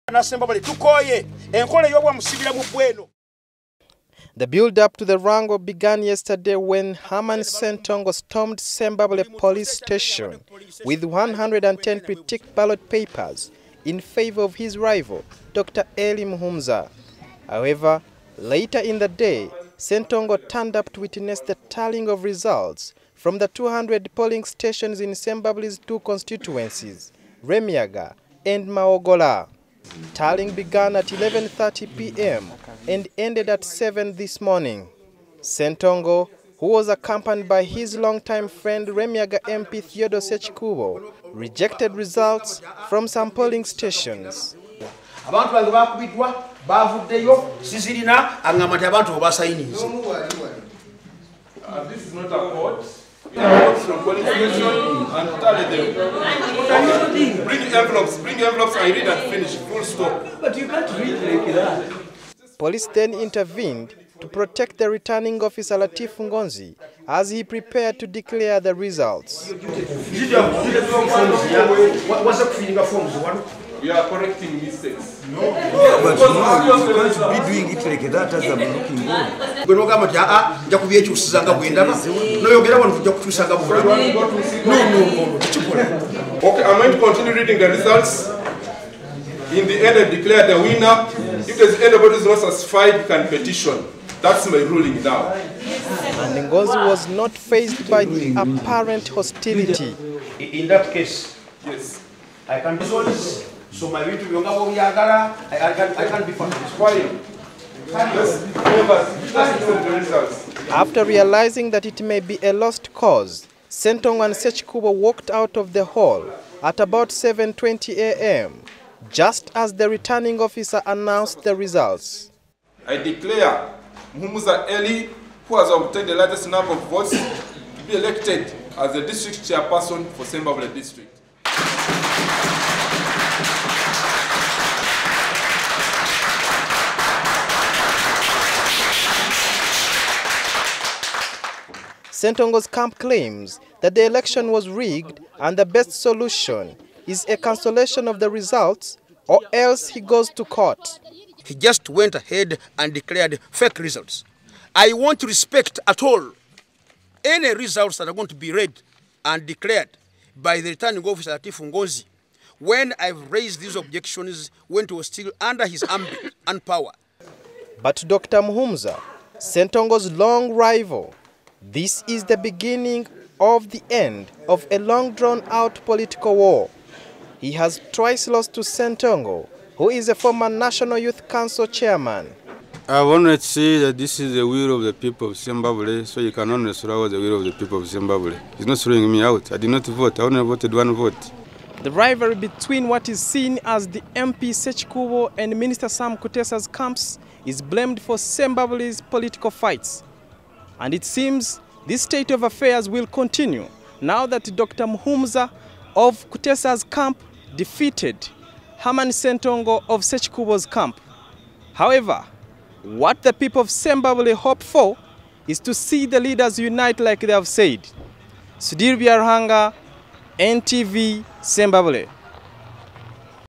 Herman Ssentongo defeated in Sembabule
Drama ensued at the Sembabule district council headquarters last night during the tallying of results for the LC5 chairmanship when two arch rivals Dr. Elly Muhumuza and Herman Ssentongo were embroiled in a war of words.